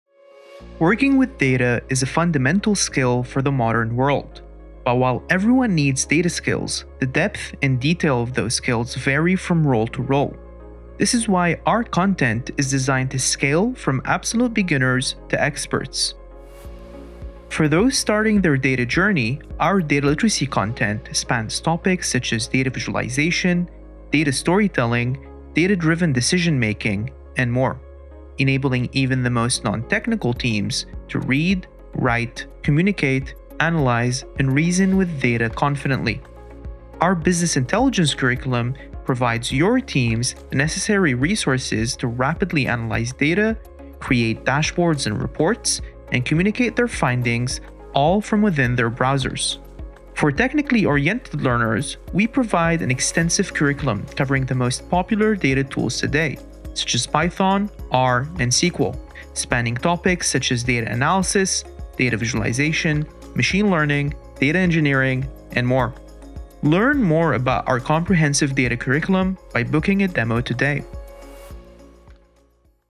Learn data skills for every role with our comprehensive curriculum | Product Demo
Data skills are essential in the modern world, but the depth and detail of those skills varies depending on your role. That's why our content is designed to scale from beginners to experts.

Whether you're a non-technical team looking to improve your data literacy, or a technically-oriented learner seeking in-depth training on popular tools like Python and R, our comprehensive curriculum has you covered. Learn more about our data courses by booking a demo today.